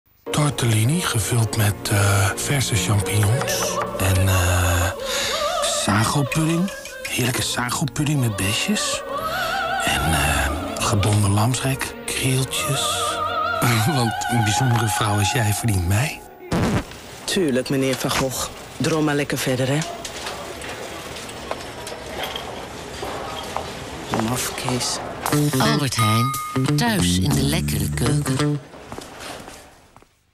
TV: Albert Heijn - Theo van Gogh en Sylvana Simons (2002) | Reclame | Reclamespot
Albert Heijn reclamespot uit 2002.

Source: VHS LP, PAL (hifi stereo).